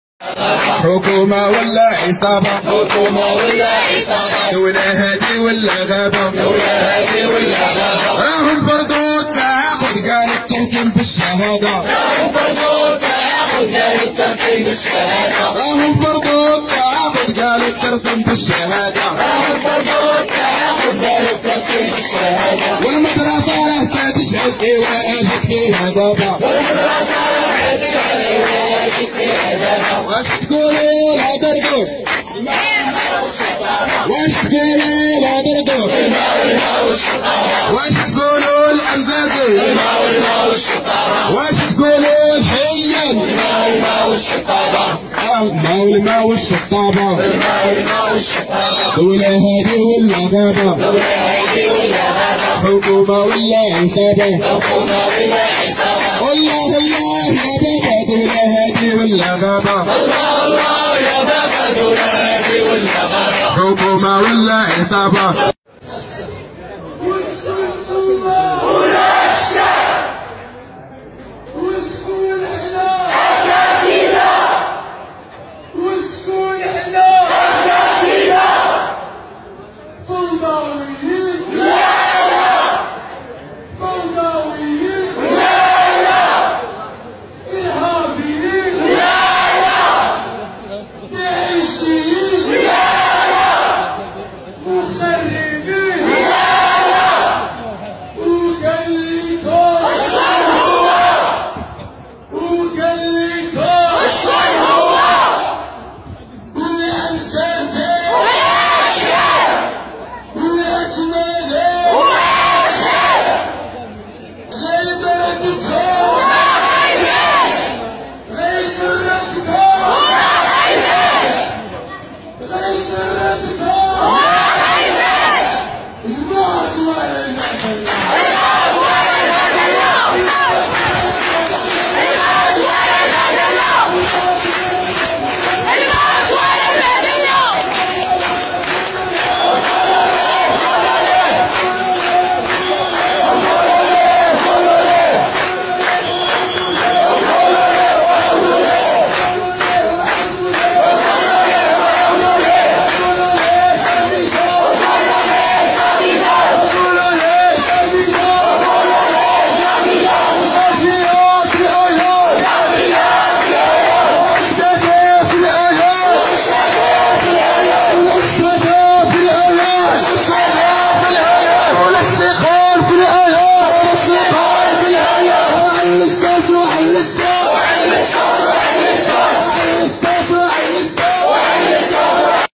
ﻮﻗﻔﺔ ﺍﺣﺘﺠﺎﺟﻴﺔ ﻭ ﺍﻋﺘﺼﺎﻡ ﻟﻴﻠﻰ ﺃﻣﺎﻡ ﻣﺪﻳﺮﻳﺔ ﺑﺮﺷﻴﺪ
ﺃﺳﺎﺗﺬﺓ ﺍﻟﺘﻌﺎﻗﺪ ﻳﺮﺩﻭﻥ ﻋﻠﻰ ﺭﻓﻊ ﺷﻜﺎﻳﺔ ﺿﺪ ﺃﺳﺘﺎﺫﺓ ﻣﺘﻌﺎﻗﺪﺓ ﺑﻮﻗﻔﺔ ﺍﺣﺘﺠﺎﺟﻴﺔ ﻭ ﺍﻋﺘﺼﺎﻡ ﻟﻴﻠﻰ ﺃﻣﺎﻡ ﻣﺪﻳﺮﻳﺔ ﺑﺮﺷﻴﺪ